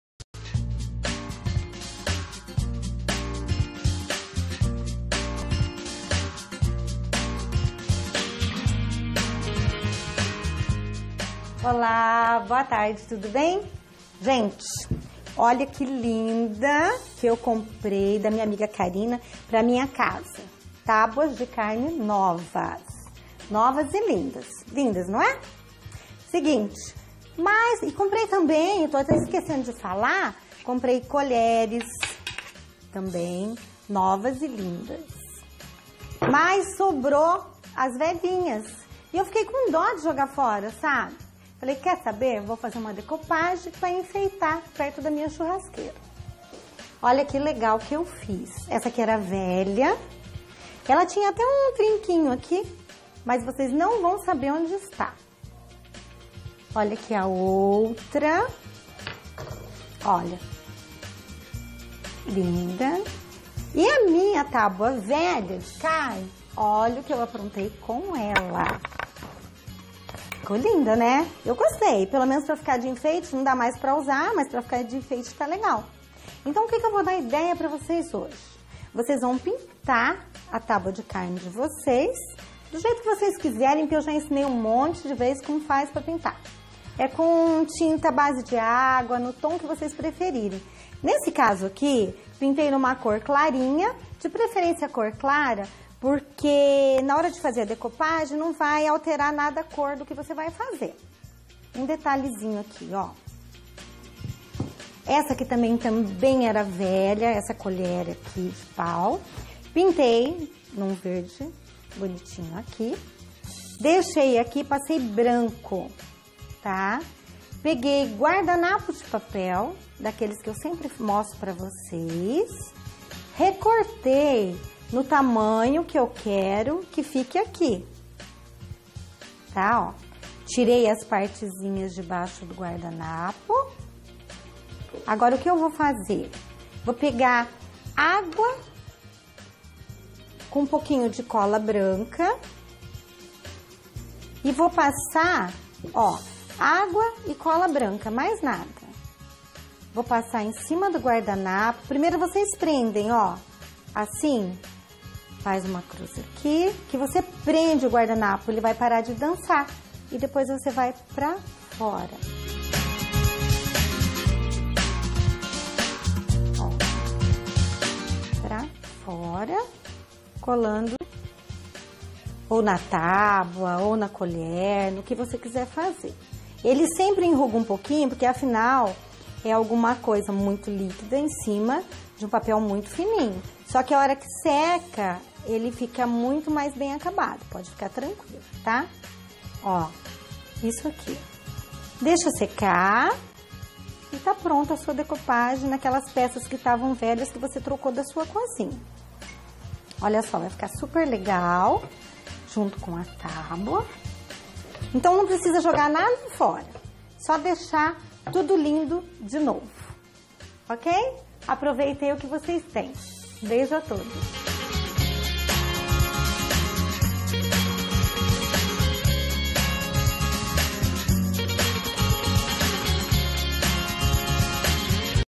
Aprenda a fazer decupagem em madeira
A artesã Ângela Santos dá dicas e ensina com o passo a passo como aplicar um guardanapo de papel decorado e transformar uma peça de madeira velha de uma forma fácil.